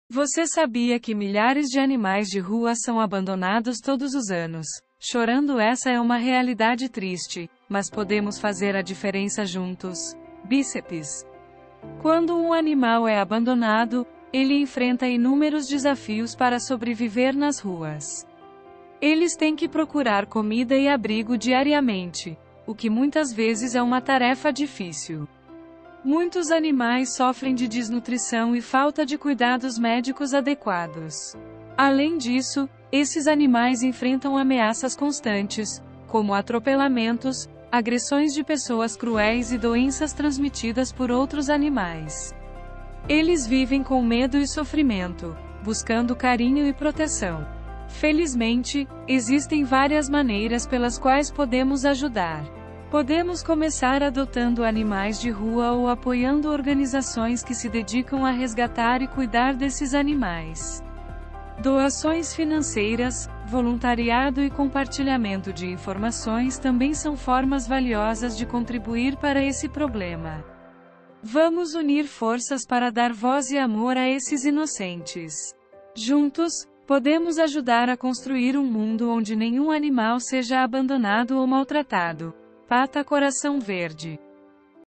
Animais abandonados
Uma breve expliocação sobre essa situação que já está virando uma tragédia não anunciada.